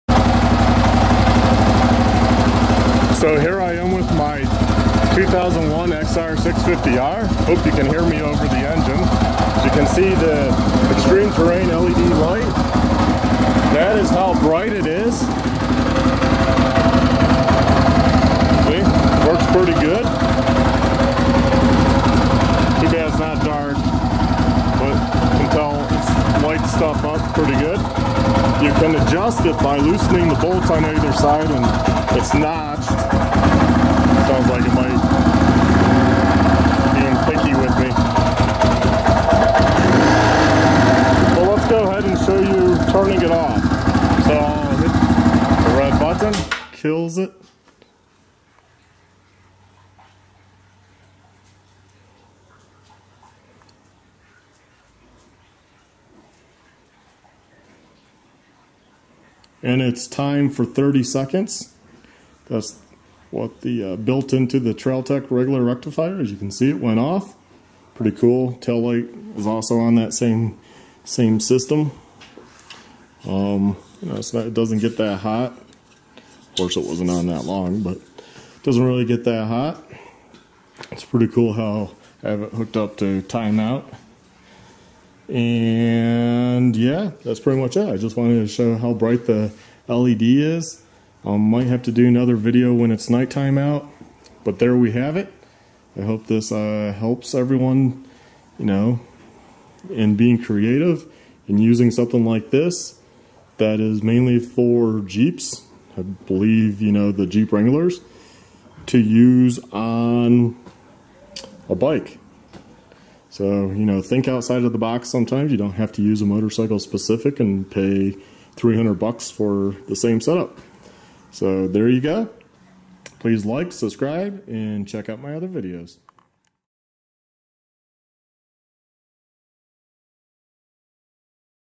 ExtremeTerrain Raxiom LED Light Bar - Part 7
Price:
$89.99

Specs:
7.5" double row LED auxiliary light
Combo Flood/Spot
(12) 3w high intensity LEDS
Output - 2100 Lumens
Color temperature - 6000K
Diecast aluminum housing with polycarbonate lens
Waterproof rating of IP 67
Dimensions 7.5" long x 3.5" wide x 3" tall
Universal application
Lifetime Limited Warranty